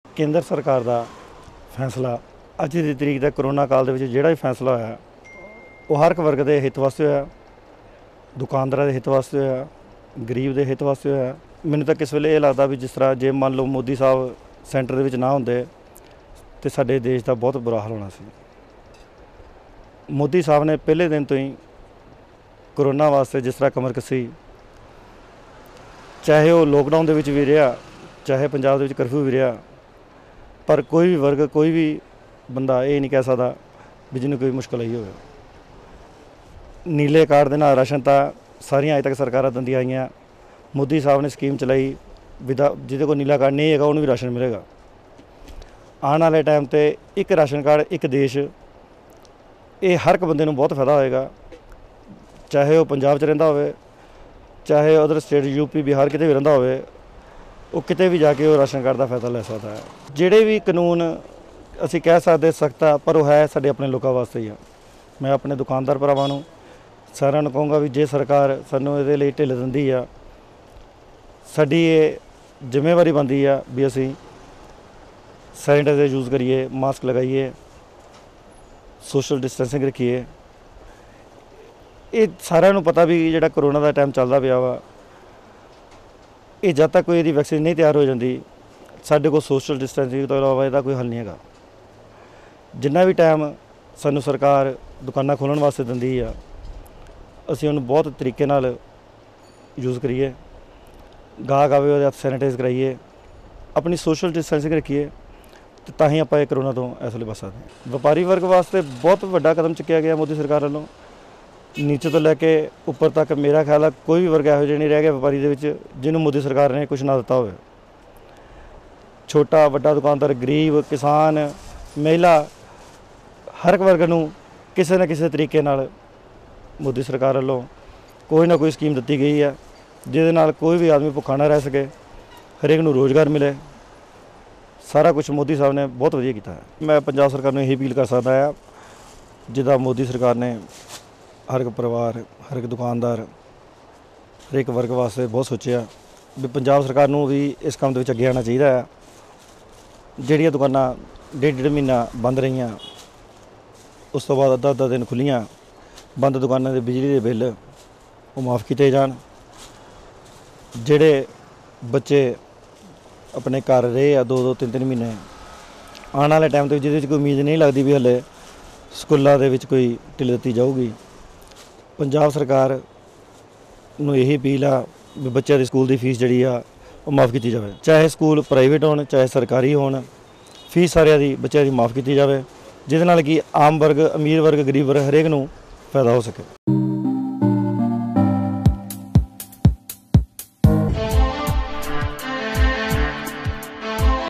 कोरोना को मिलकर हराना है : दर्पण गुप्ता।
जिला भाजपा होशियारपुर व्यापार सैल के प्रधान दर्पण गुप्ता ने कोरोना वायरस से बचाव हेतु जनता से की अपील, सरकार द्वारा दी गई छूट में हिदायतों का करें पूर्ण रुप से पालन, घर से बाहर निकलने से पहले मुंह पर मास्क लगाना और बार-बार हाथ धोना न भूलें, कोरोना को मिलकर हराना है और पुन: स्वस्थ्य समाज बनाना है: दर्पण गुप्ता।